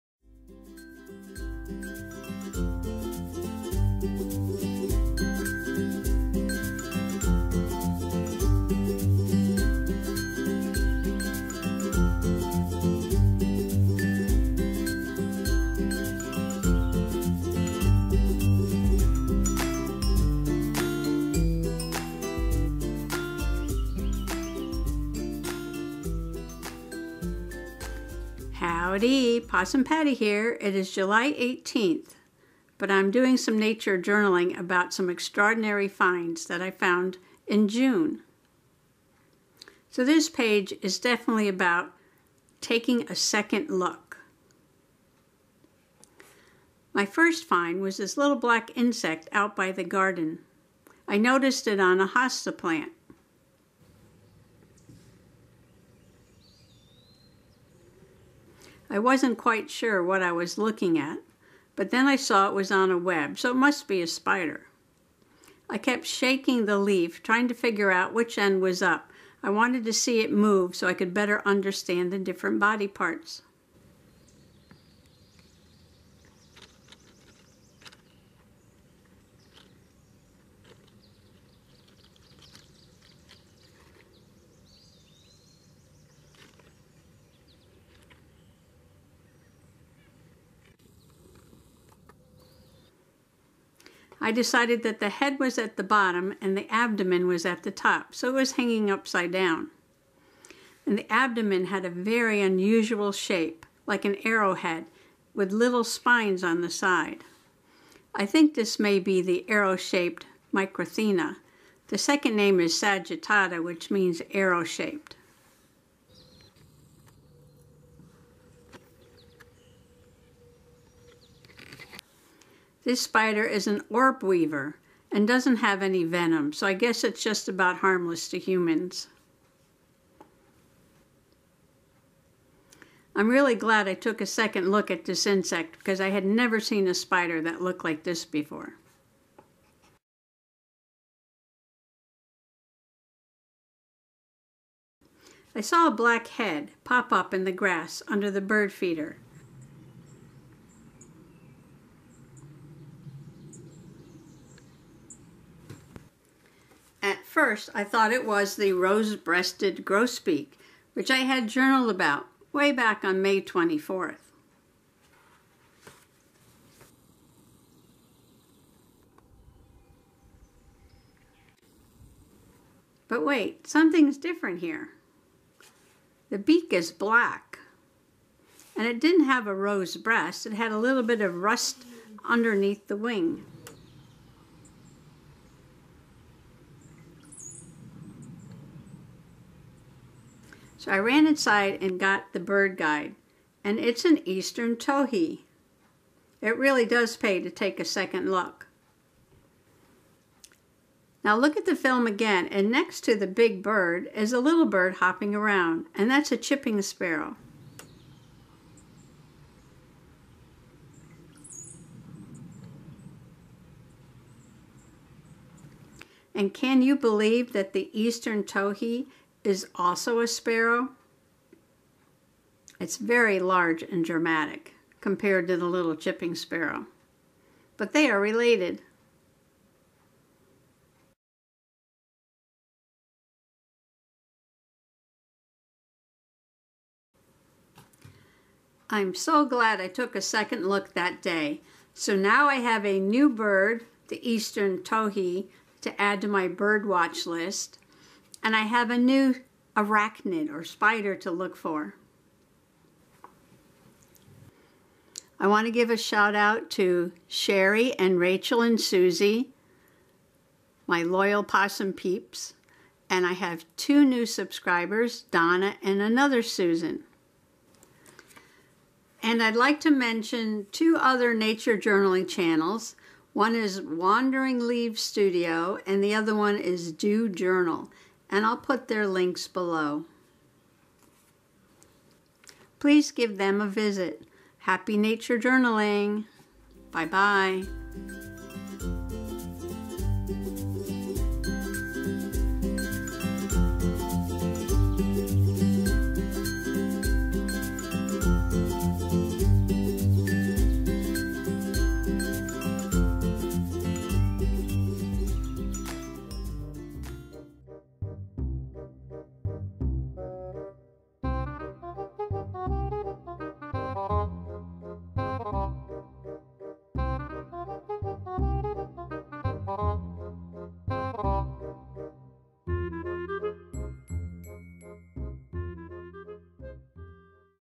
NATURE JOURNAL It Pays to Take a Second Look! 2 New (to me) Species!
Come along with me, Possum Patty, as I investigate 2 new species! The first, I mistook for an insect and the second, a Rose-breasted Grosbeak. I was wrong both times!